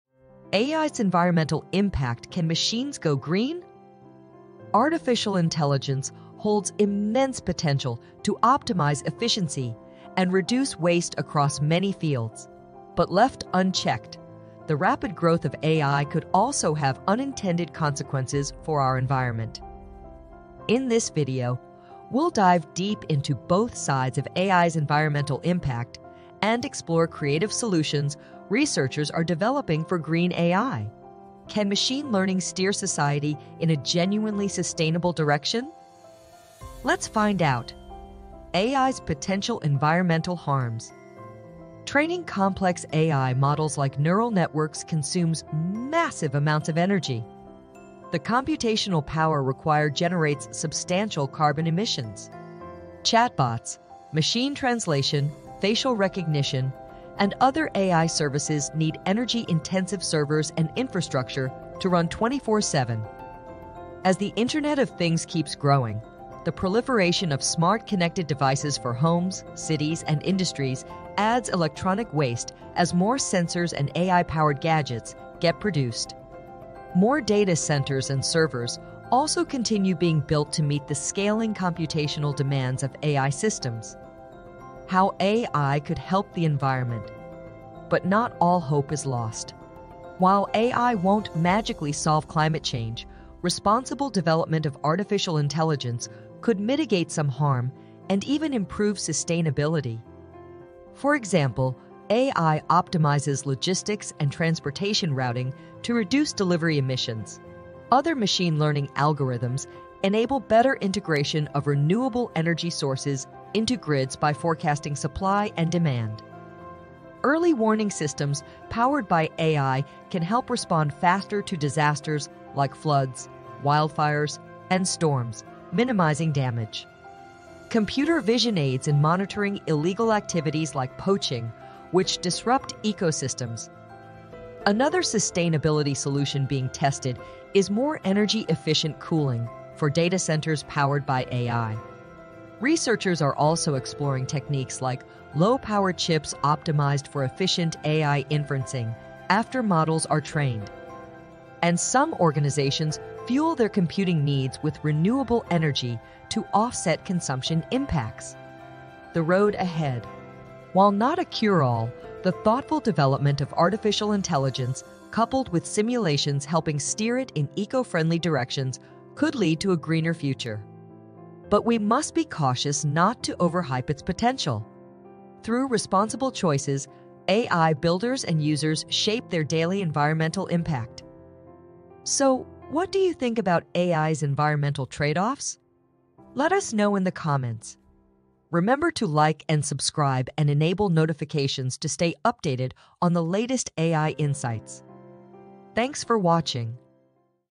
AI's Environmental Impact - Can Machines Go Green?
AI's rapid growth brings immense possibilities but also environmental concerns. This thought-provoking video explores whether artificial intelligence can curb its harms and steer humanity toward sustainability.

We examine both sides of AI's environmental impact - from its massive energy demands and electronic waste to its potential for optimizing renewables and preventing ecological disasters. Can green machine learning develop into more than an oxymoron?

Journey through the complex debates to gain a balanced perspective on what responsible AI could achieve for our planet's future health and limitations. Learn about promising innovations researchers are pioneering - from energy-efficient neural network chips to AI tracking systems modeling environmental footprints in real-time.

Hear insights on AI's role in sustainability from leading experts across technology, business, and environmental fields. Gain an unprecedented understanding of this emergent, high-stakes issue through engaging analysis.

Will AI's continued growth damage our world or redeem itself by enabling climate-saving breakthroughs? Watch this impartial deep dive to find out!